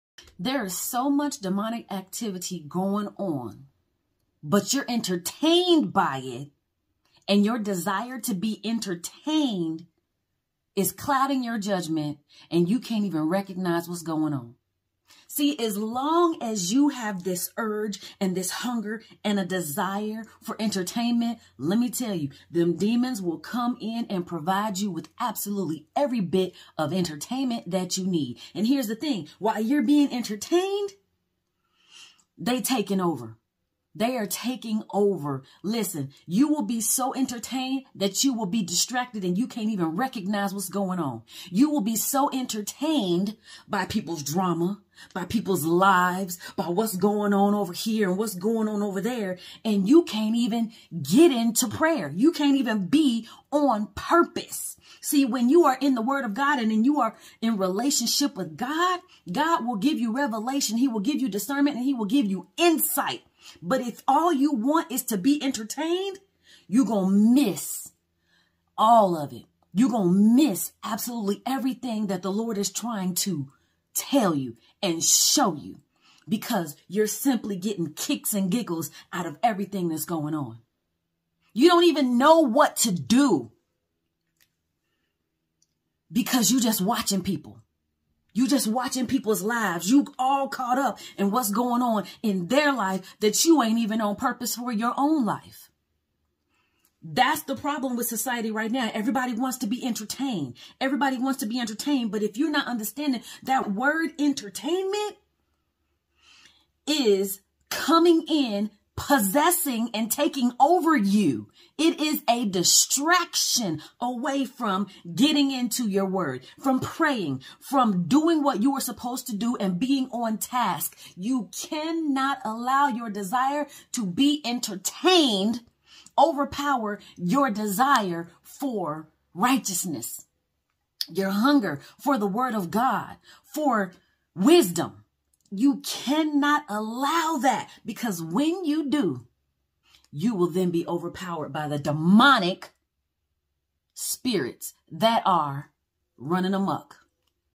Distractions on purpose!
If all you want is "entertainment," the demons will provide you with plenty of it, so that you'll be so distracted that you won't be on purpose- praying and getting into the Word of GOD! You'll be so busy watching everyone & everything!